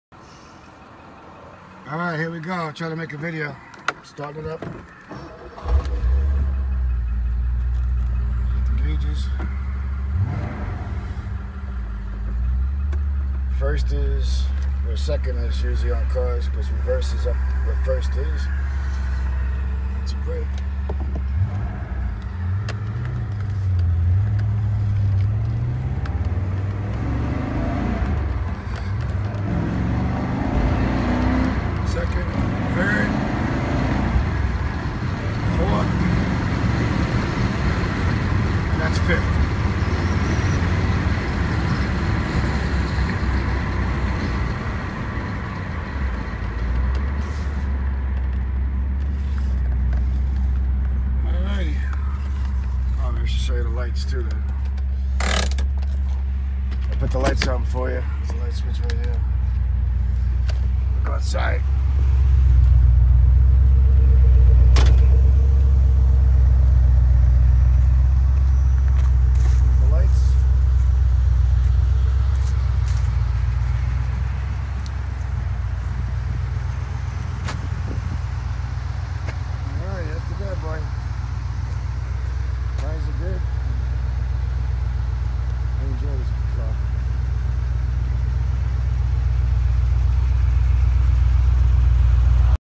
$17,995 - 1979 Porsche 928 For Sale~RARE 5 Spd~A/C~Runs Great
1979 Porsche 928 For Sale

Real nice classic and well taken care of 928 with the rare 5 speed option. Runs and looks perfect and everything works including ice cold air conditioning. No leaks or smoke and real fast! Recent professional re-paint in Porsche gray metallic. 105k miles. Upgraded 18 inch wheels are on car now but original phone dial wheels come with it. All services have been done including new timing belts, clutch, brakes, battery and complete tune up. Binder with all records comes with car and Original Wheels Included in the Sale.